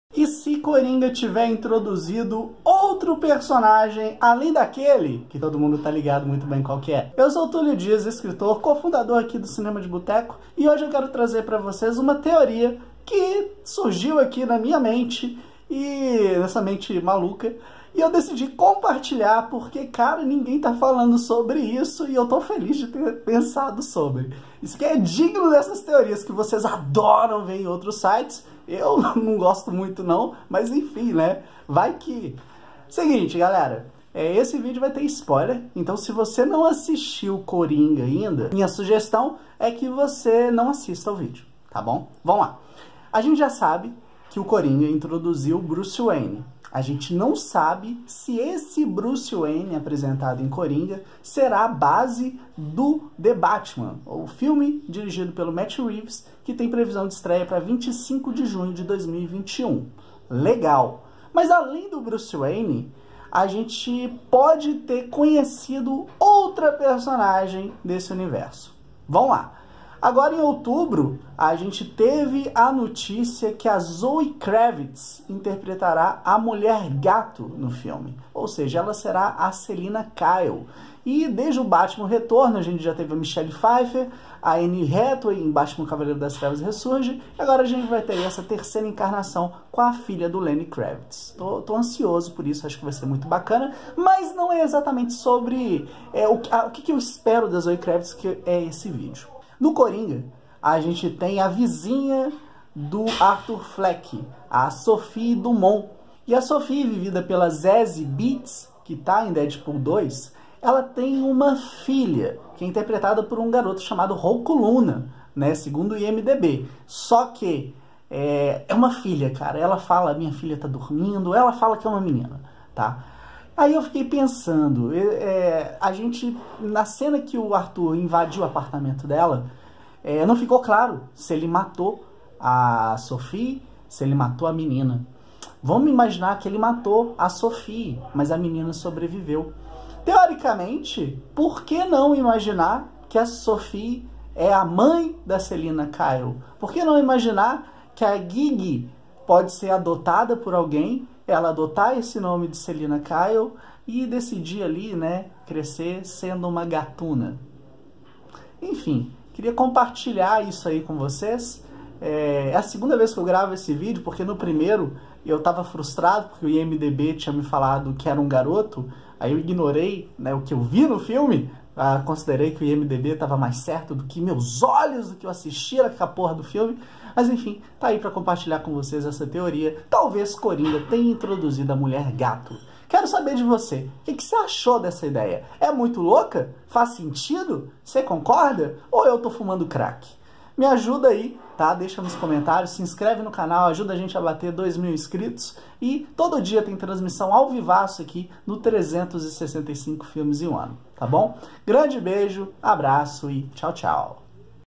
E Se Coringa Tiver Apresentado OUTRO Personagem Além DAQUELE...
Teorias: E Se Coringa Tiver Apresentado OUTRO Personagem Além DAQUELE... Bem, eu não vou entrar no campo dos spoilers aqui porque não quero estragar experiência de ninguém, mas faz muito sentido considerar que SE Coringa não acabar ignorado na linha temporal de The Batman, de Matt Reeves, ele introduziu mais que um personagem importante...

O que você acha?

Edição: Juliana Lacerda